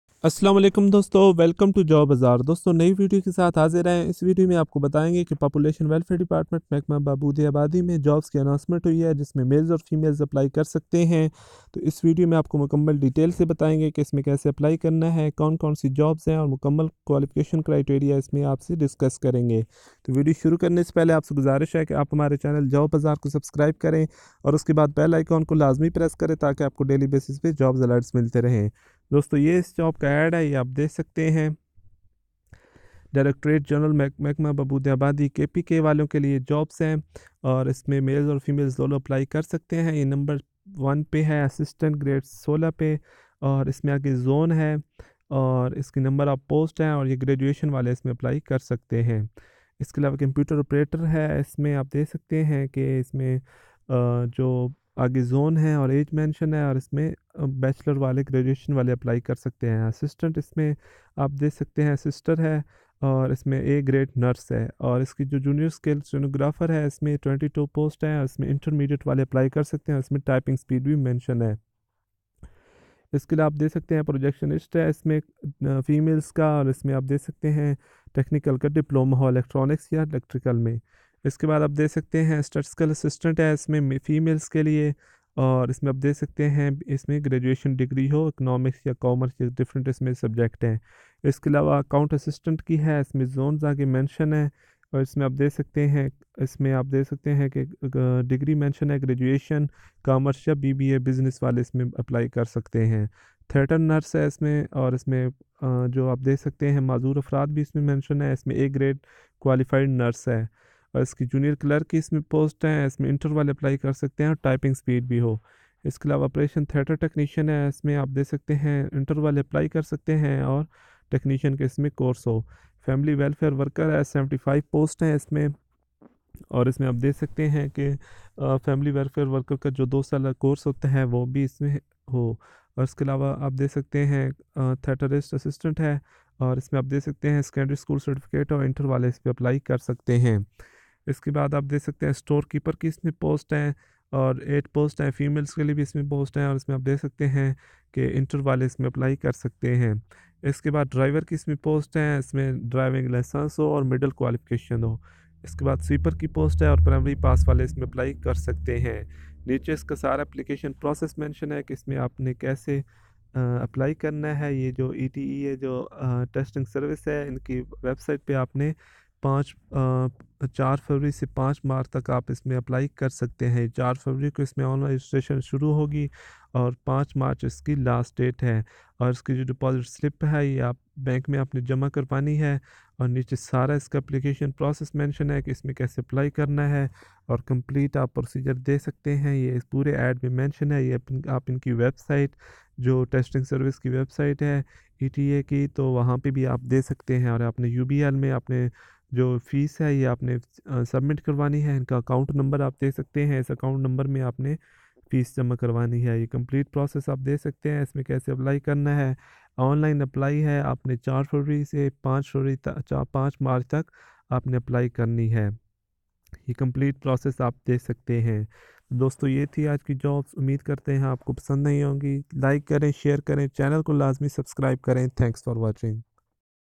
Population welfare department jobs 2019 | Multiple jobs for males and females
Hello everyone
Population welfare department kpk announce multiple jobs for males and females of kpk. In this video u can get complete detail of this job.